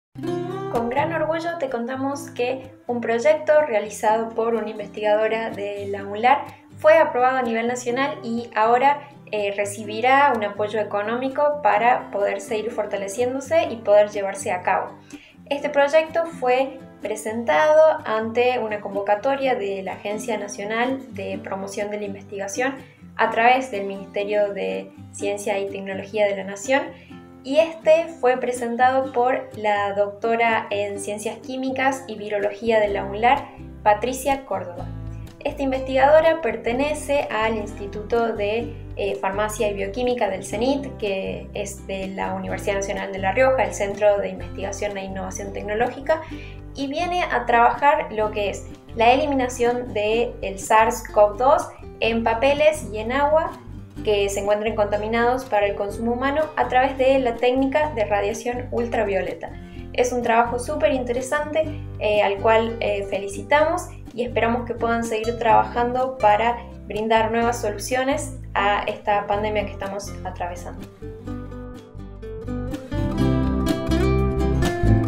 Docente de la UNLaR seleccionada por Nación para investigar sobre COVID - 19
La Dra. en Ciencias Químicas y Virología, Patricia Córdoba, investigará la eliminación del SARS COV2 en papeles y en agua con radiación ultravioleta. Natalie Leautier lo explica con más detalle.